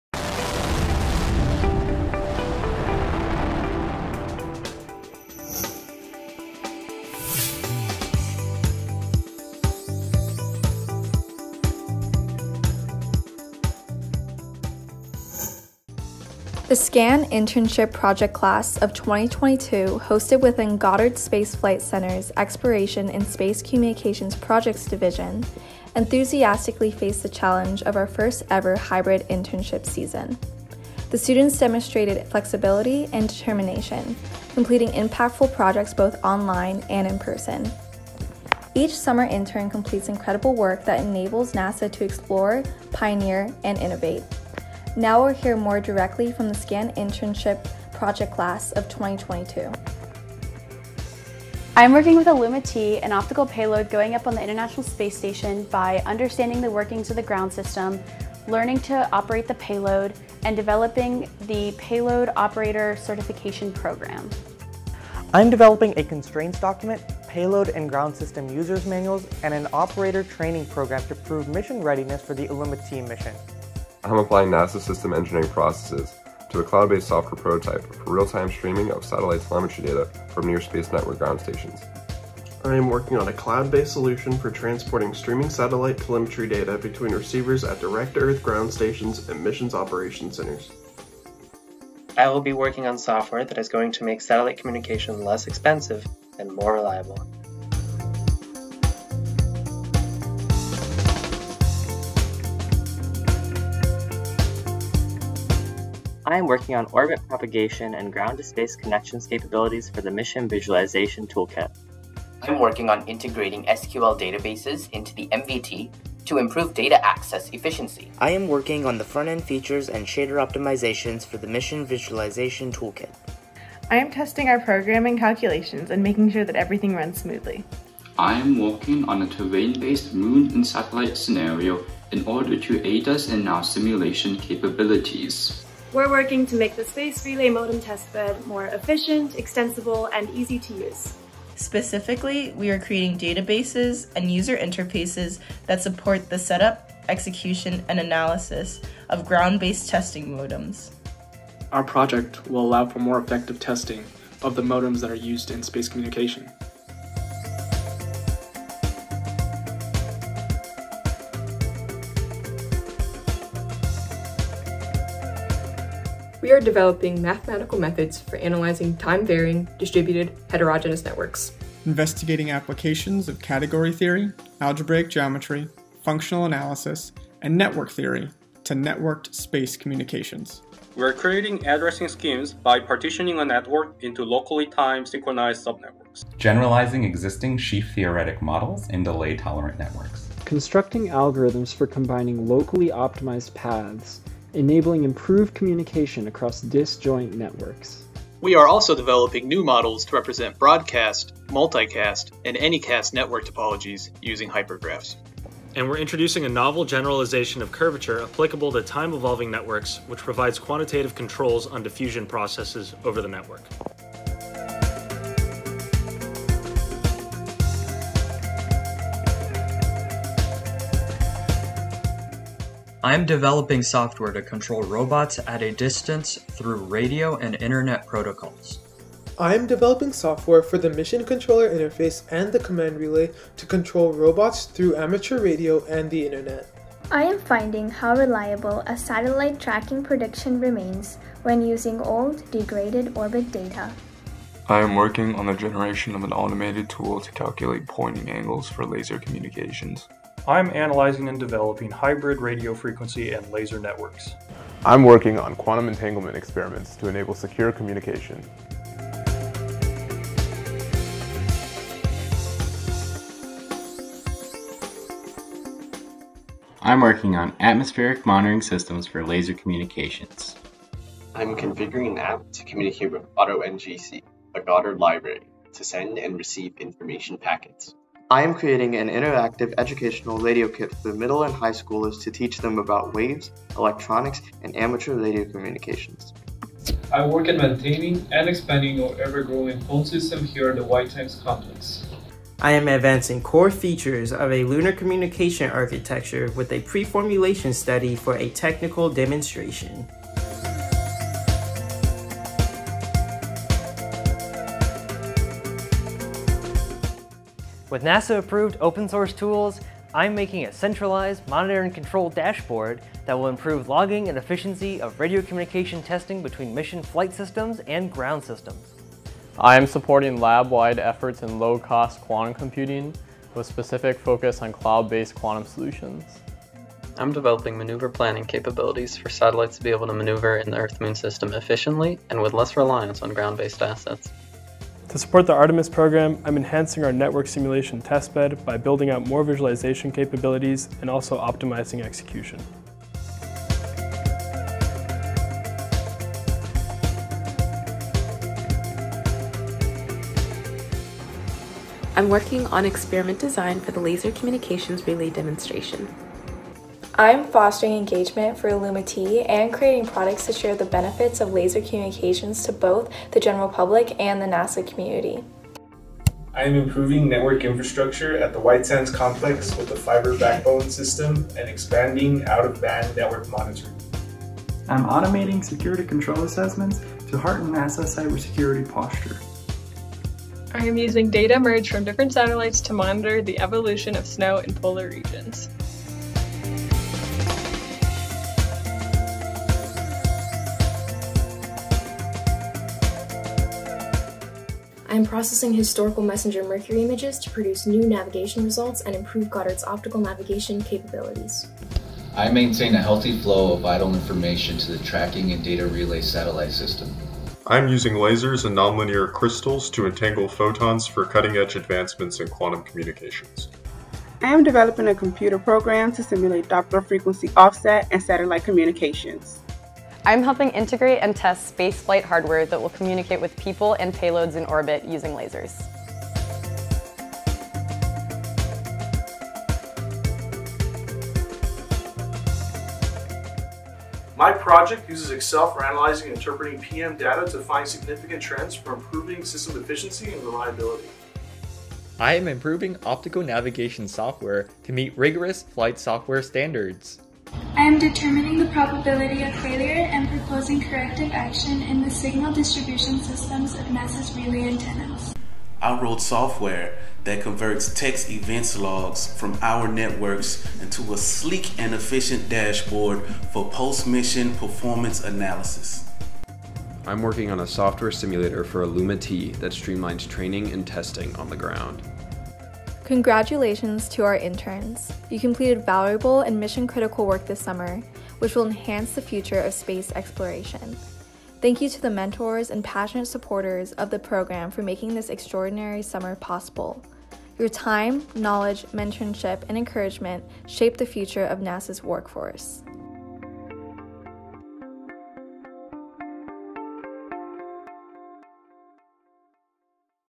SCaN Internship Project 2022 (NASA Goddard)
Meet the talented students of the Space Communications and Navigation (SCaN) Internship Project (SIP). In Summer 2022, 54 interns and externs from across the country accepted the challenge of a fully remote internship with the team at NASA's Goddard Space Flight Center. They brought their diverse personal and educational backgrounds to NASA to enhance the future of space communications and navigation. Learn more about their projects and the amazing work they accomplished!

Apply to the SCaN Internship Project through intern.nasa.gov (search for ‘SCaN’)